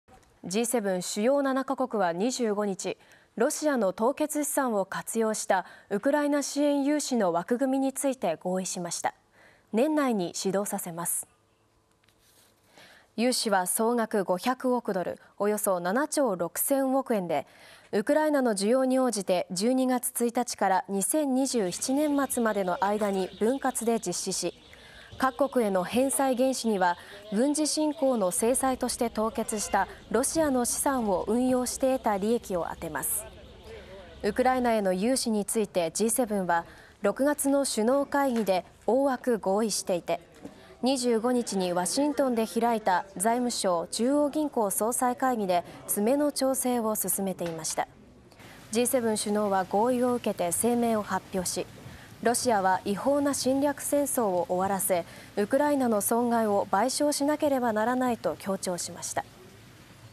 G7 総額500億ドルのウクライナ融資枠組みで合意 返済原資にロシア凍結資産を活用
G7＝主要7カ国は25日、ロシアの凍結資産を活用したウクライナ支援融資の枠組みについて合意しました。年内に始動させます。融資は総額500億ドル＝およそ7兆6,000億円で、ウクライナの需要に応じて12月1日から2027年末までの間に分割で実施し、各国への返済原資には、軍事侵攻の制裁として凍結したロシアの資産を運用して得た利益を充てます。ウクライナへの融資についてG7は、6月の首脳会議で大枠合意していて、25日にワシントンで開いた財務相・中央銀行総裁会議で詰めの調整を進めていました。G7首脳は合意を受けて声明を発表し、「ロシアは違法な侵略戦争を終わらせ、ウクライナの損害を賠償しなければならない」と強調しました。